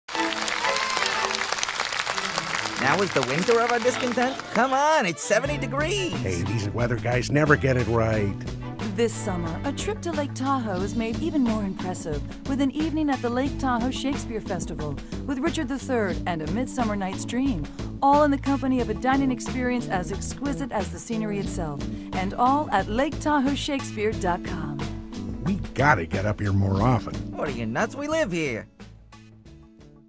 TV Spot for Lake Tahoe Shakespeare Festival 2008
This summer, a trip to Lake Tahoe is made even more impressive with an evening at the Lake Tahoe Shakespeare Festival, with "Richard the Third," and "A Midsummer Night's Dream," all in the company of a dining experience as exquisite as the scenery itself.  And all...at LakeTahoeShakespeare.com.